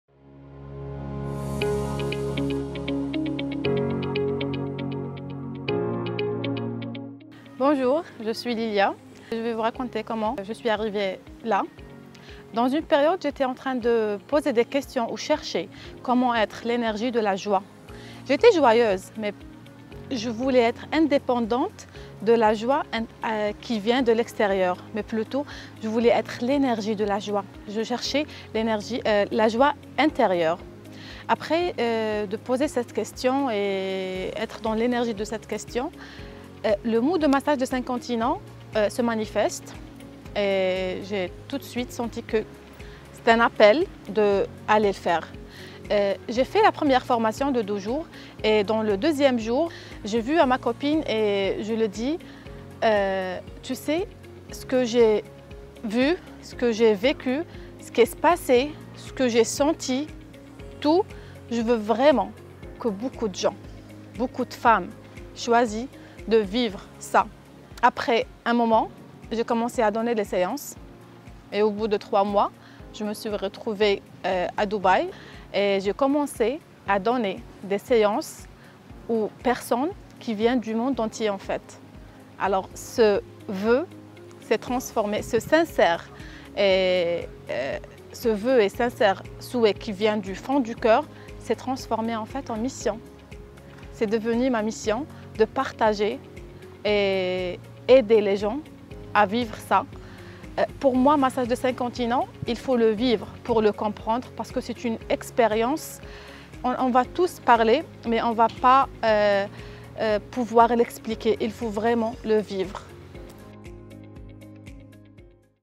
Success Story - Massage des 5 Continents - Lilia Tounsi
Un magnifique témoignage sur son expansion personnelle et professionnelle avec le Massage des 5 Continents ! Tout ce bonheur expansé à travers tes formations de ce soin à DUBAÏ !

Gratitude pour ces transmissions de joie à travers le monde entier